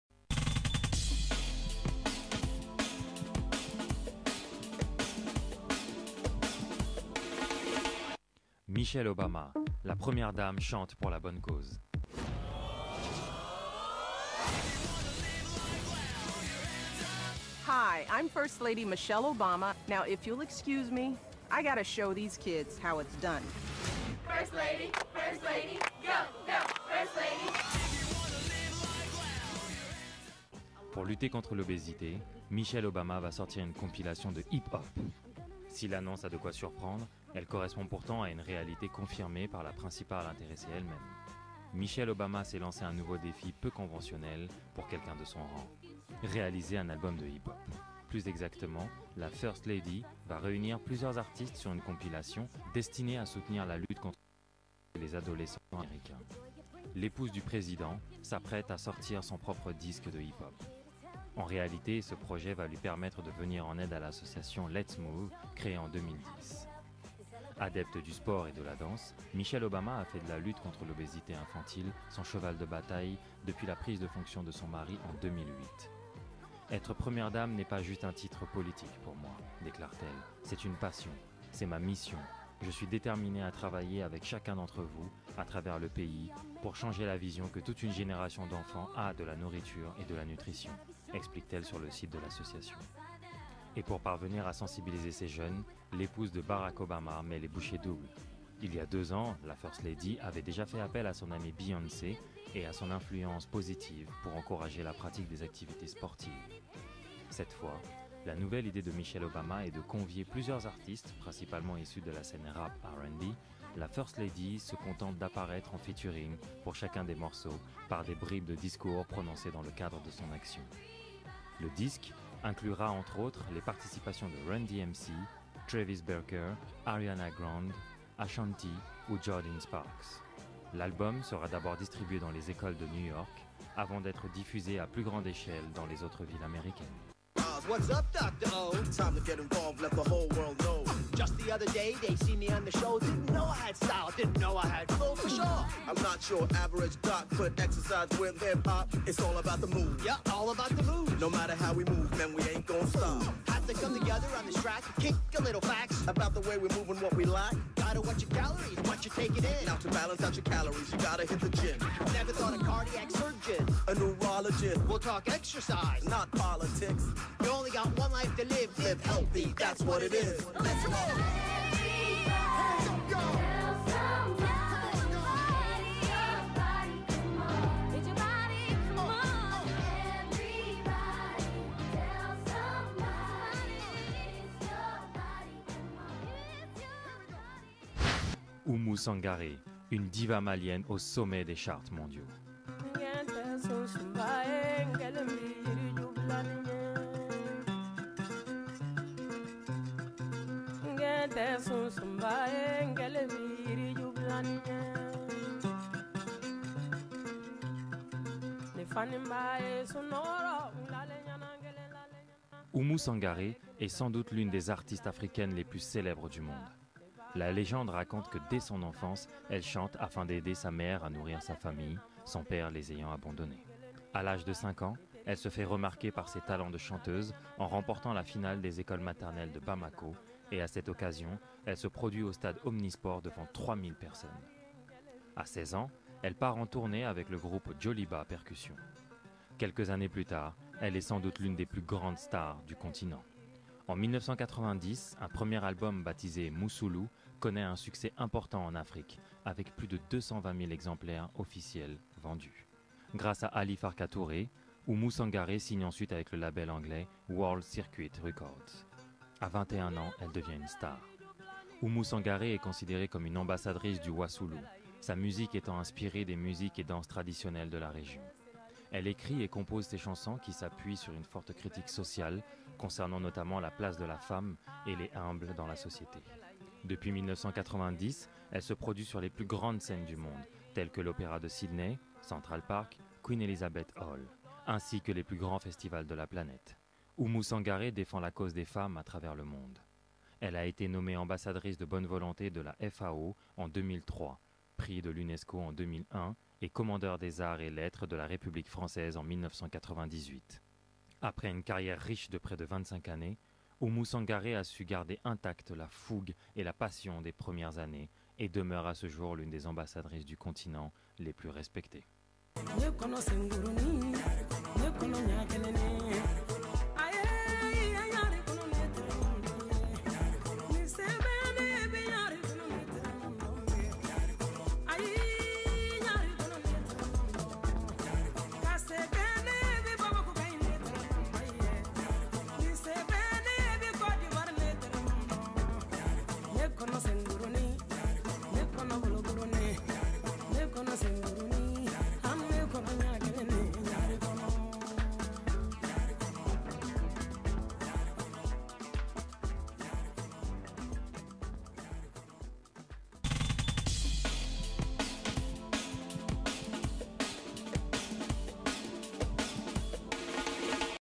UBIZNEWS / Le JT du Showbiz du Jeudi 11 Juin 2015 avec Michelle Obama
Vous découvrirez dans cette édition du JT du Showbiz comment la première Dame des USA, Michelle Obama,  utilise la musique pour promouvoir la pratique du sport dans son pays. A NE PAS RATER !